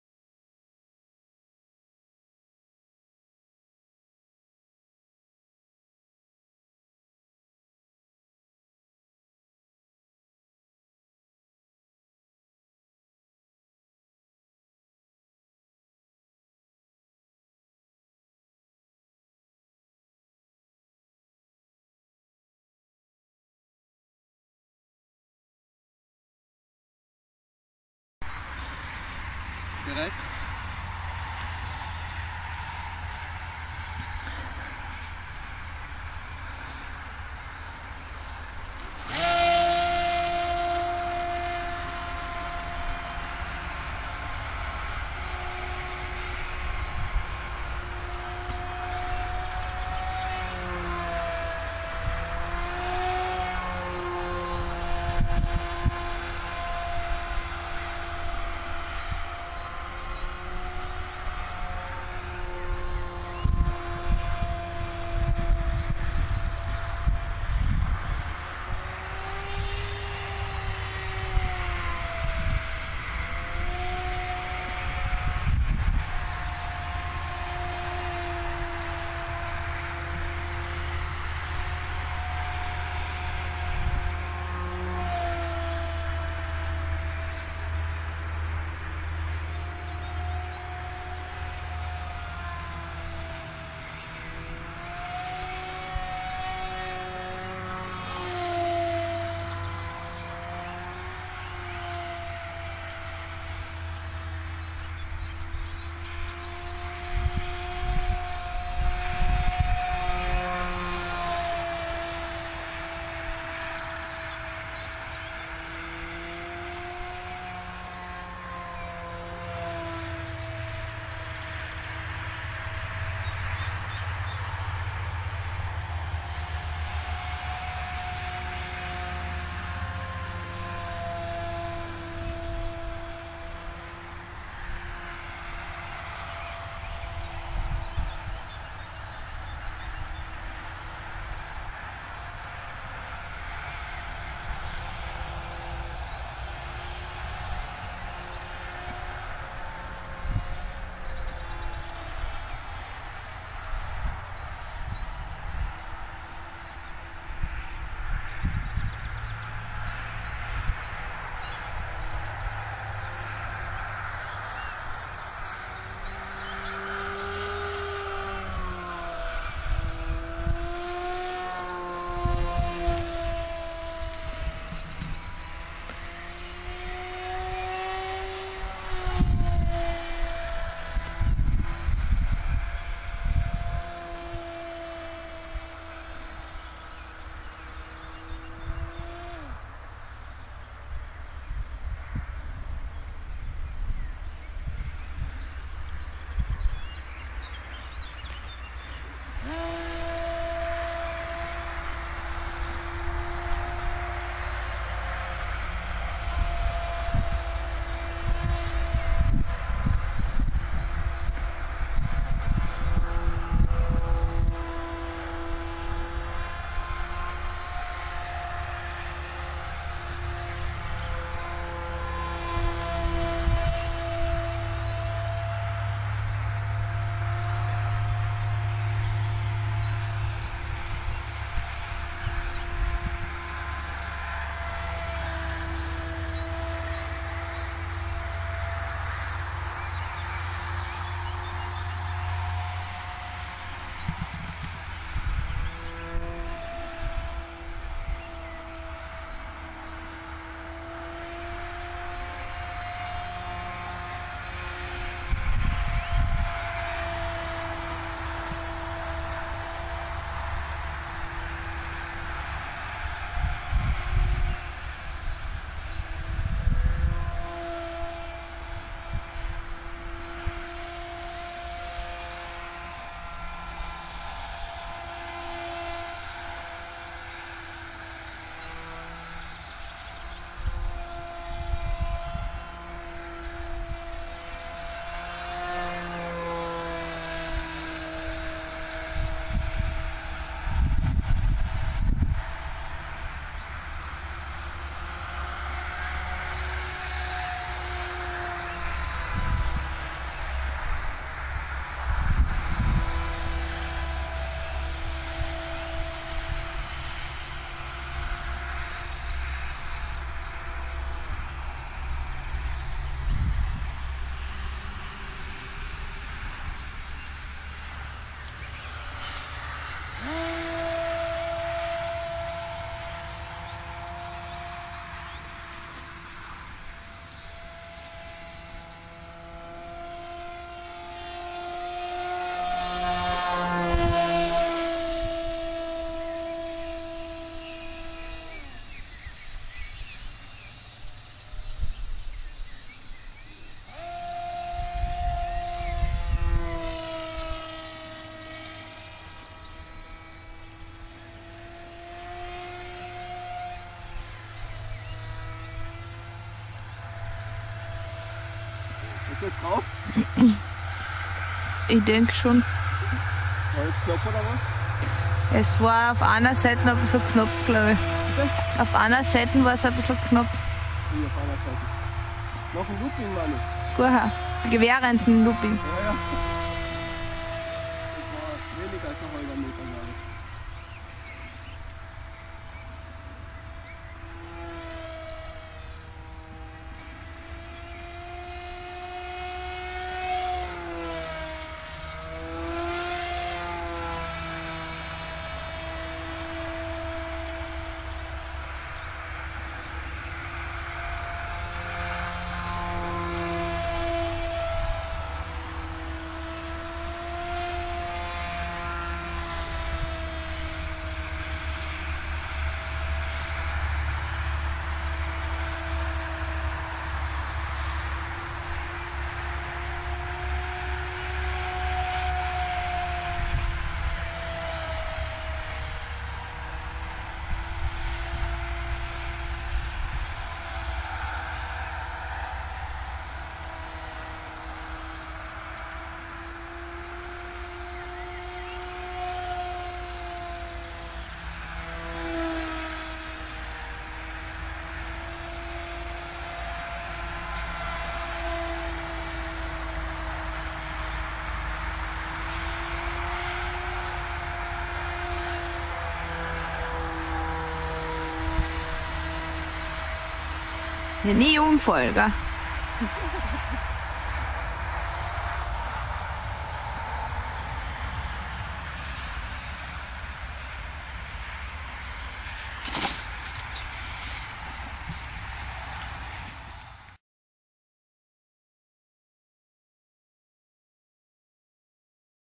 Maiden Flight of my self designed Flying Wing (RC Model Plane)
Michael Ransburg